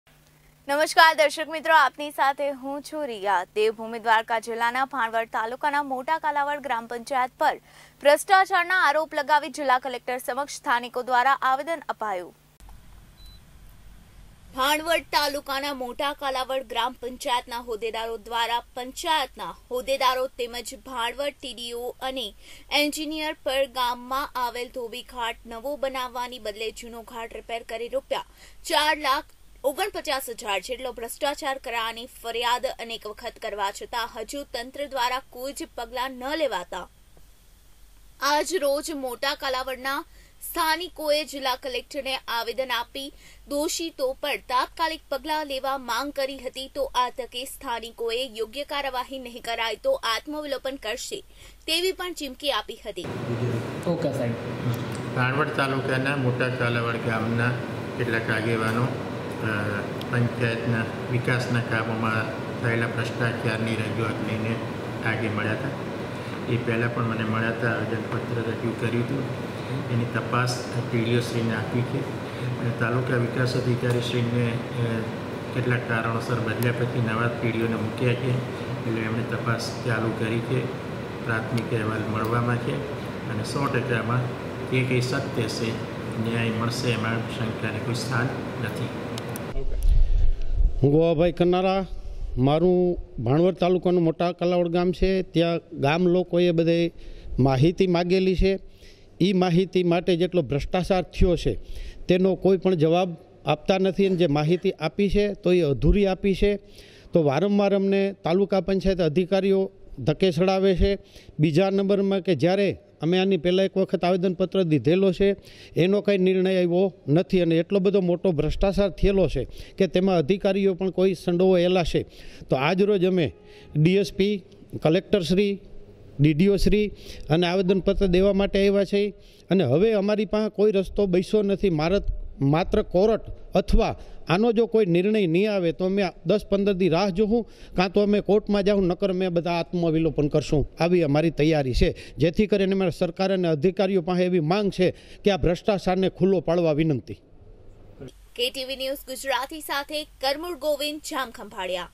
દેવભૂમિ દ્વારકા જિલ્લાના ભાણવડ તાલુકાના મોટા કાલાવડ ગ્રામપંચાયત પર ભ્રષ્ટાચારના આરોપ લગાવી
દેવભૂમિ દ્વારકા જિલ્લાના ભાણવડ તાલુકાના મોટા કાલાવડ ગ્રામપંચાયત પર ભ્રષ્ટાચારના આરોપ લગાવી જિલ્લા કલેકટર સમક્ષ સ્થાનિકો દ્વારા આવેદન અપાયું